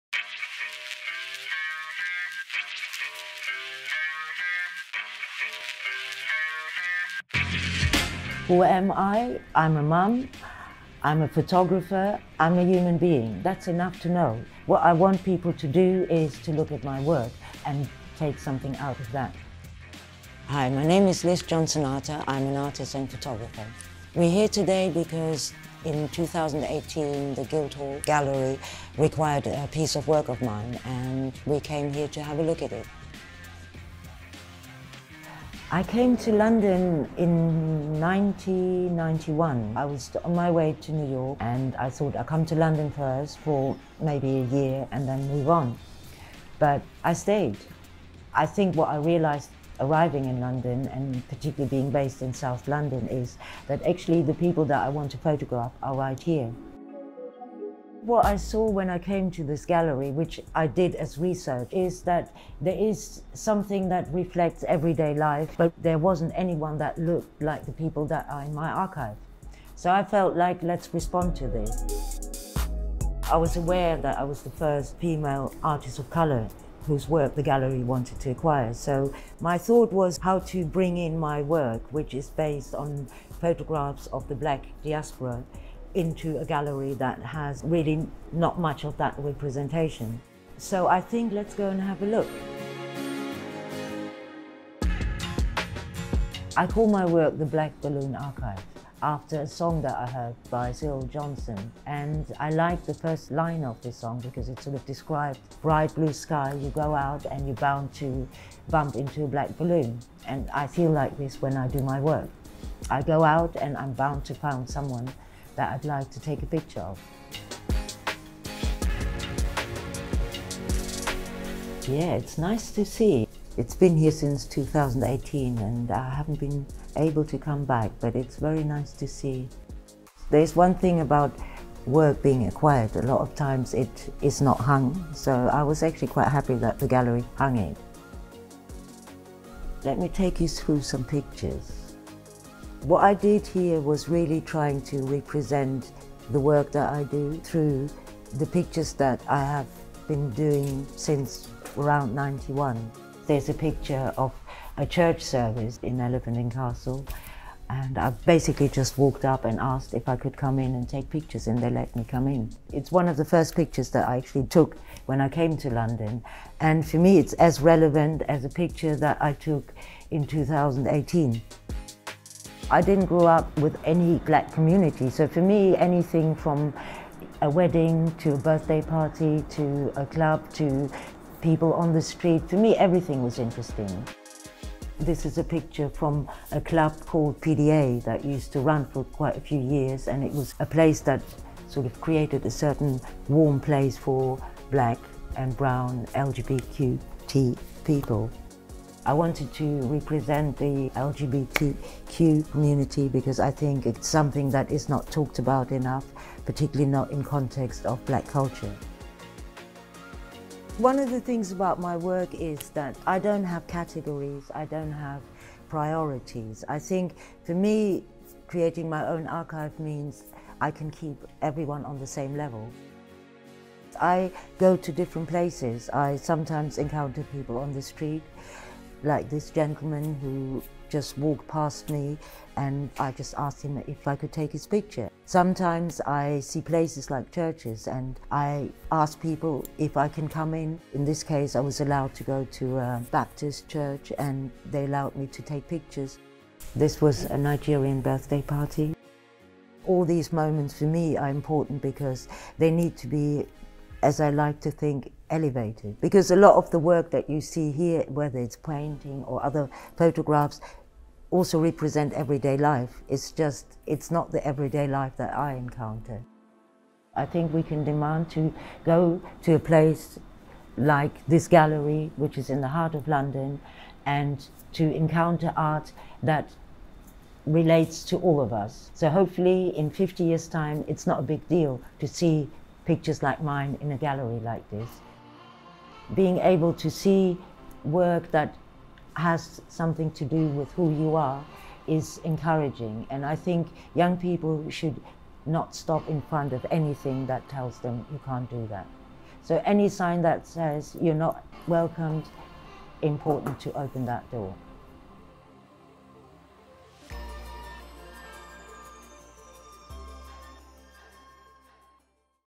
Our Stories: Reflecting on Black British History in Four Objects | Guildhall Art Gallery
This short film is part of Culture Mile Learning's Black History Month Project for October 2020, ‘Our Stories: Reflecting on Black British History in Four Objects’.

In this film, photographer Liz Johnson Artur discusses her artwork ‘Top Shelf’ at Guildhall Art Gallery.